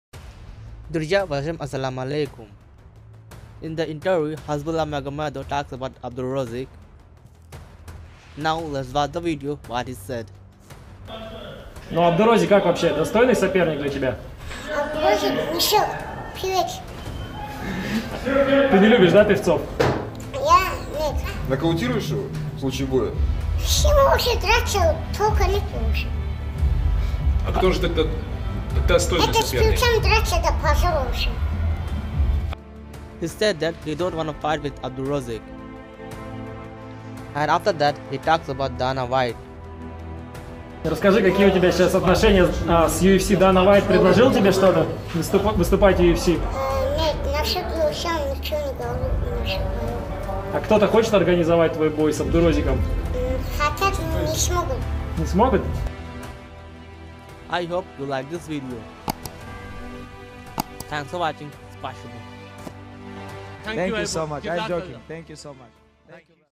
Hasbulla Magomedov talks about his fight against "Abdu Rozik"
In This video Hasbulla Magomedov talks about Abdu rozik, and about Dana White!